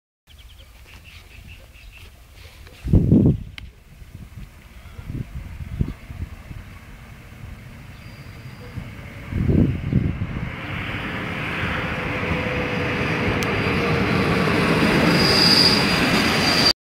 362.010-1 na Os4608
ZSSK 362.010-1 prichádza do žst.Tvrdošovce ako Os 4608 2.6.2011
kamera: Denis K.